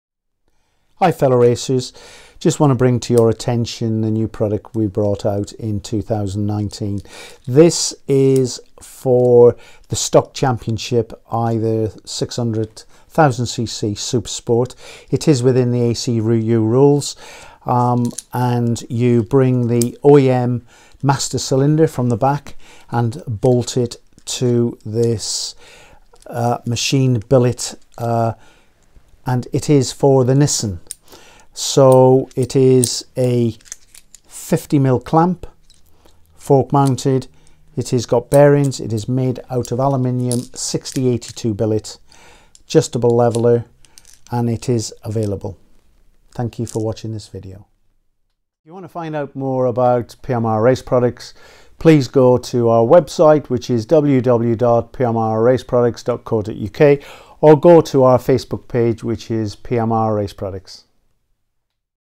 Stock Thumb Brake for use with OEM (Nissan)
The pivot point and the pivot point of the handle adjustment are both double bearing, which eliminates the friction of the max. 25 bar pressure in the fulcrum. This results in a lesser force with the fingers to get the same brake pressure. More importantly, the feeling improves so that the rear brake is easier to control.

The thumb brake gives great advantages for better driving style, resulting in faster lap times. With the thumb you have a better braking feeling than with the foot.

Furthermore, you can now also use the rear brake in a right turn when it is difficult to operate the rear brake with the right feeling.